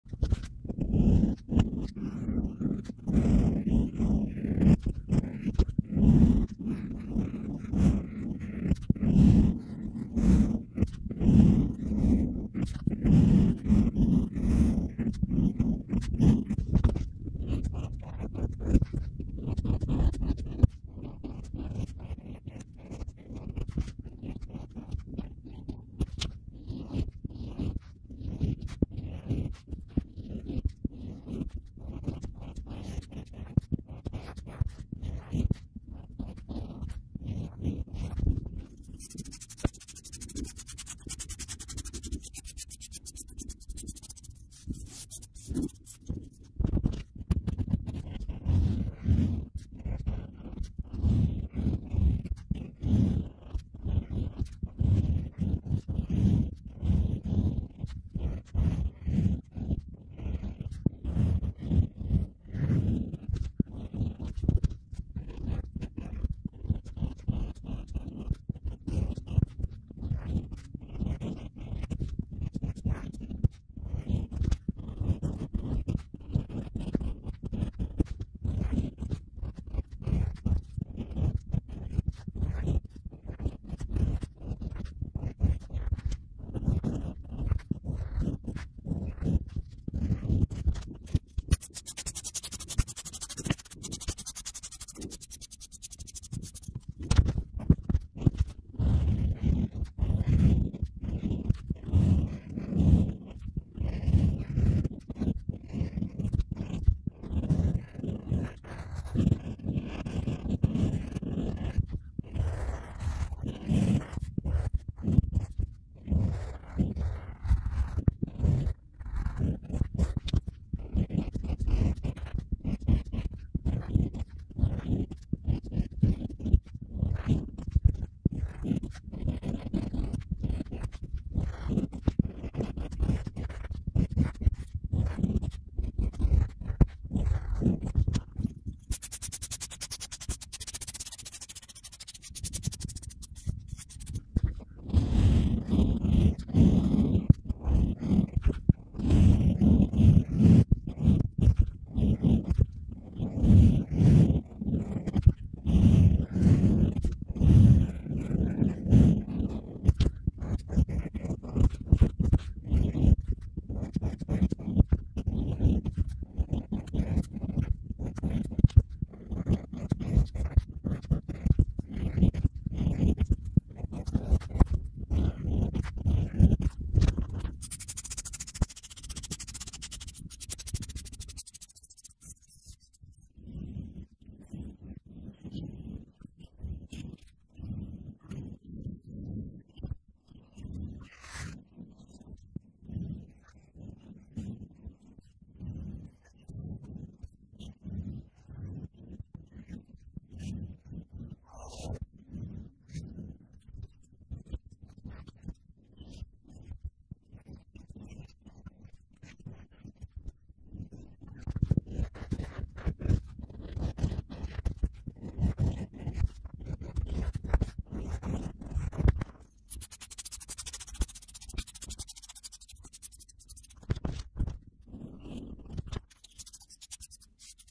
Very Satisfying ASMR - Tik Tok Name Compilation
Clay Carving & Painting ASMR: Enjoy the nice, sandy, crunchy sounds from AlexaJaneStudios.

Q & A:

I am carving into clay! This is ceramic, water based pottery clay that hasn’t been fired yet. I am using a stylus tool to create the design in the wet clay.

What if you mess up?
If it’s not too big of a mistake, I will try to add it into the design. If I mess up really bad, I add the clay into a recycle bucket where I can turn the clay back into a clay that is ready to be thrown or molded again.

How do I get into Ceramics?
I highly recommend finding a local studio/university where you can take some classes! You can learn on your own, but there is a lot of information to learn from how to throw, working with the clay, firing the kiln, mixing glazes, proper safety, etc. Lots of work, but lots of fun!